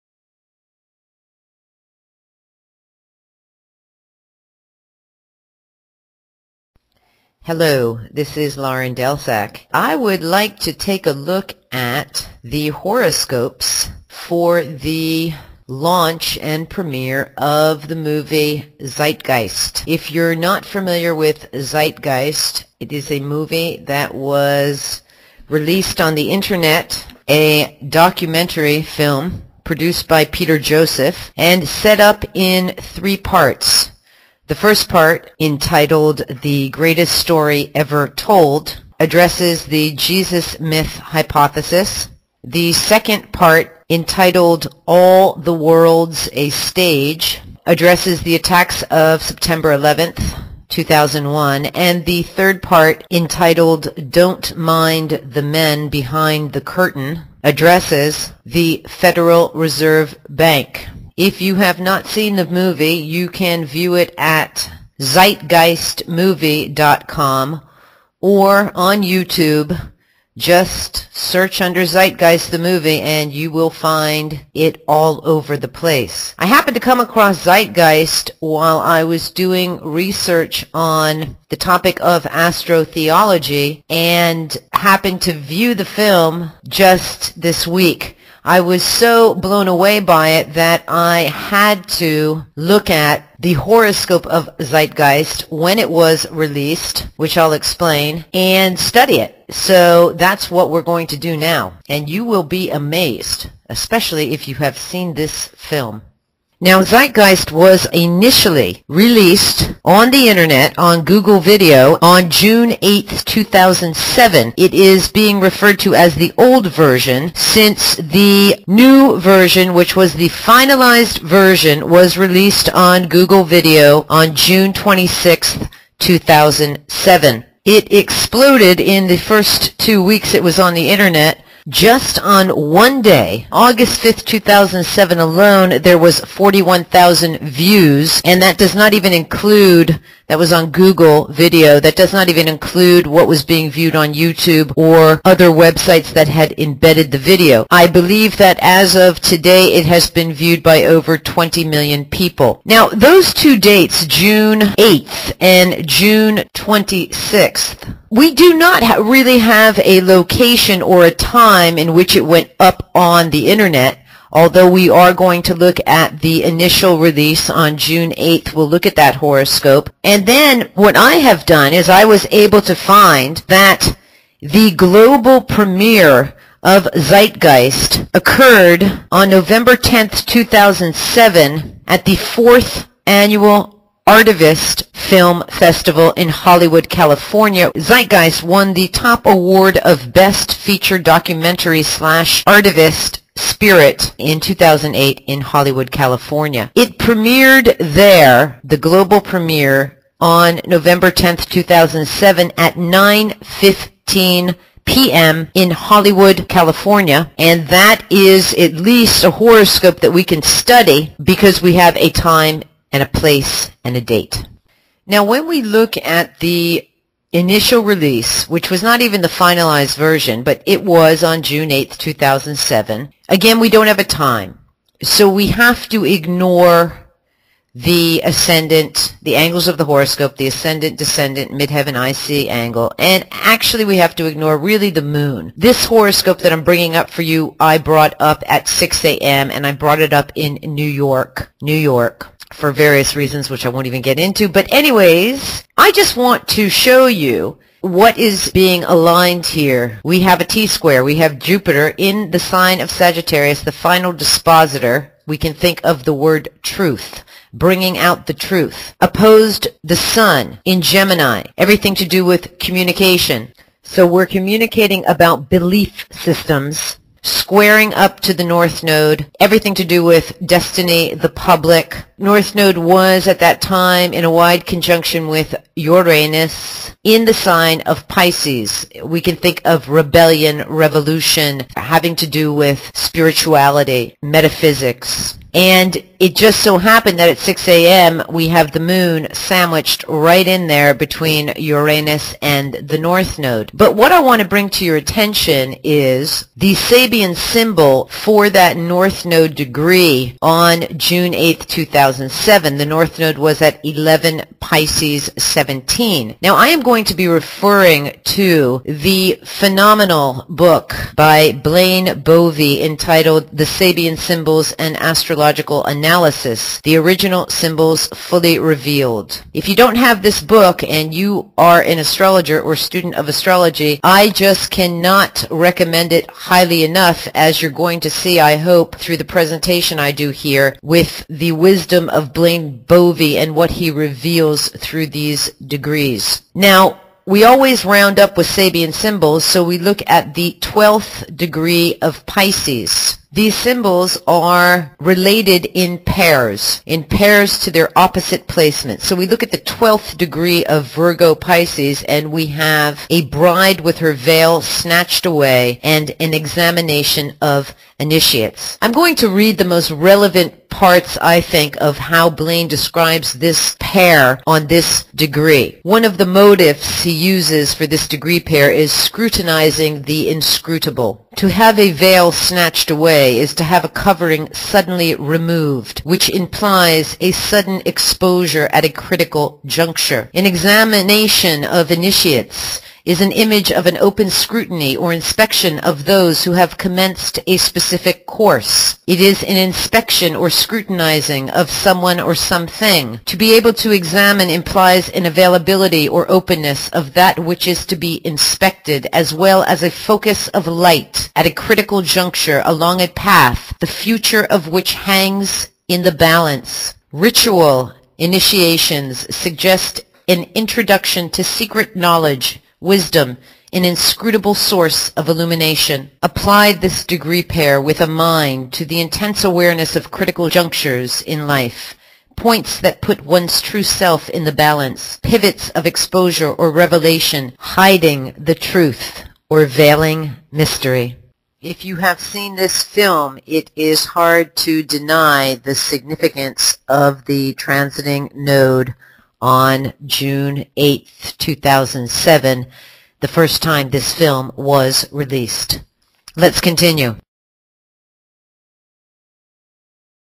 Astrology: The Horoscopes for Zeitgeist The Movie (Part 1 of 3)
Astrology Part 1: A study of the horoscopes for Zeitgeist The Movie: its first appearance on the internet, its global premiere at the 4th Annual Artivist Film Festival, and incorporating the draconic chart in synastry with the event chart.

Thank you, Lauren